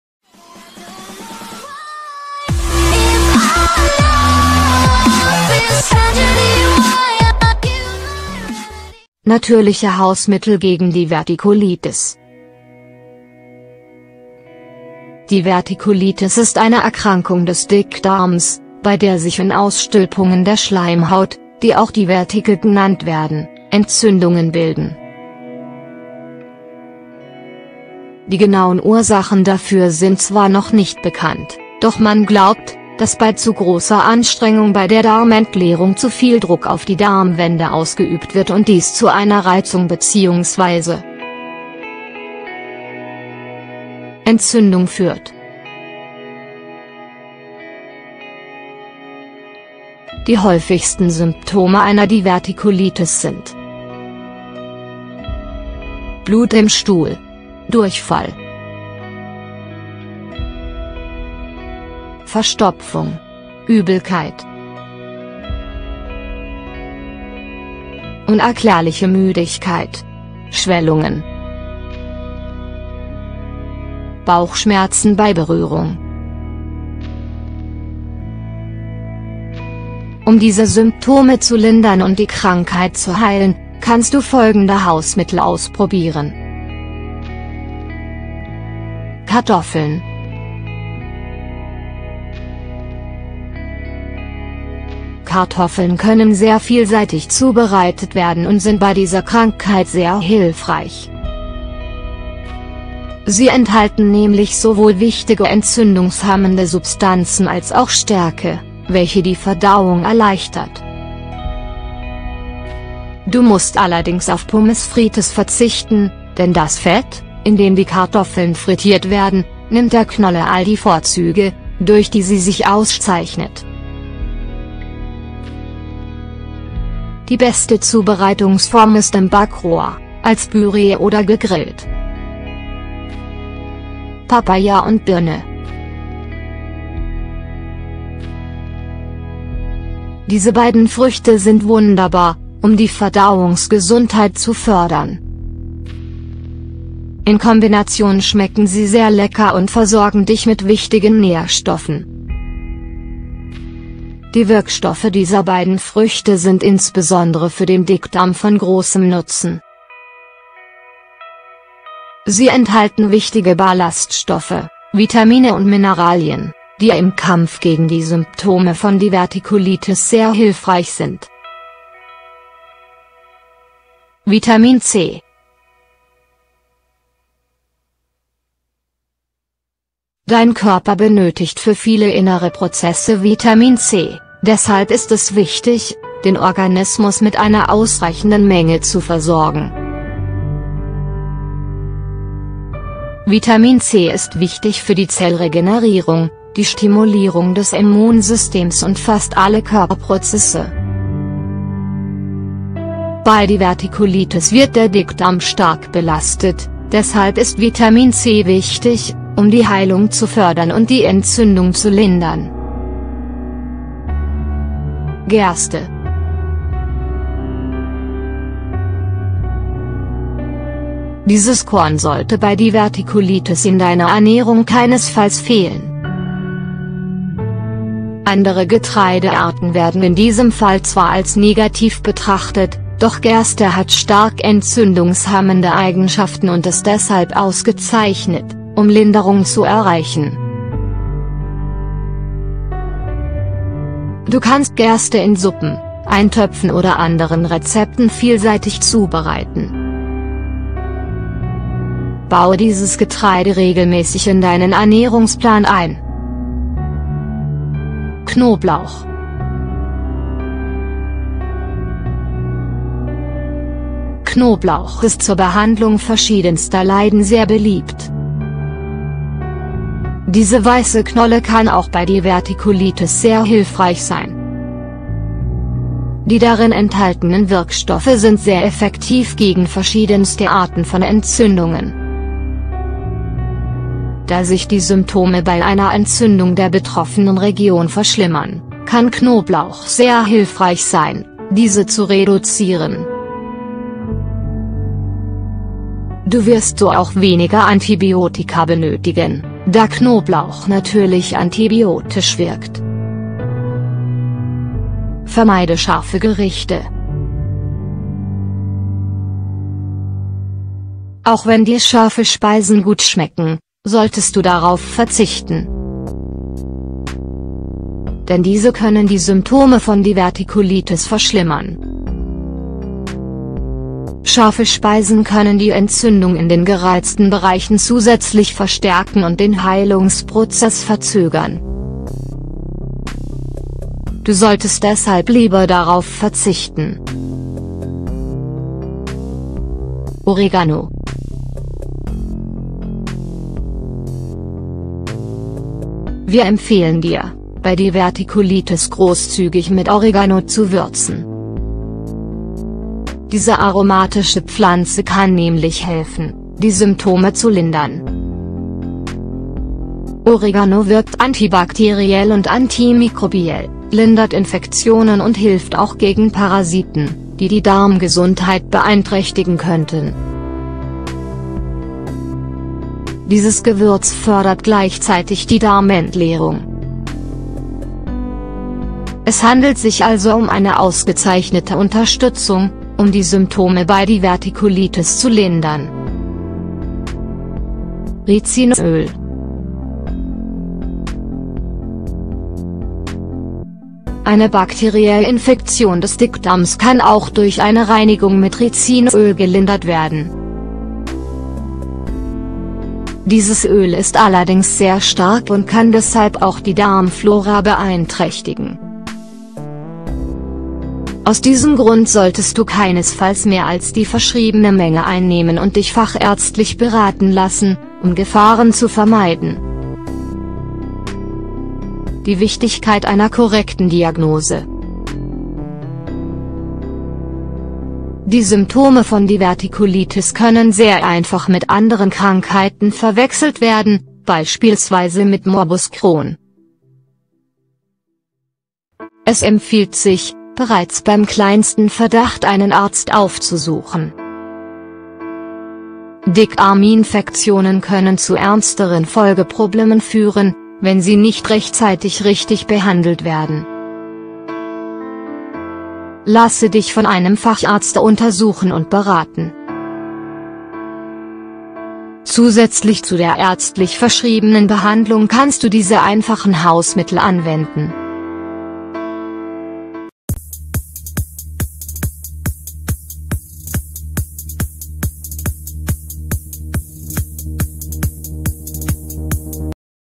Natürliche Hausmittel gegen Divertikulitis
Beim geringsten Verdacht auf Divertikulitis solltest du einen Facharzt aufsuchen, der eine Diagnose stellen und gegebenenfalls die richtige Behandlung einleiten kann. Zusätzlich kannst du die Beschwerden durch die richtige Ernährung lindern.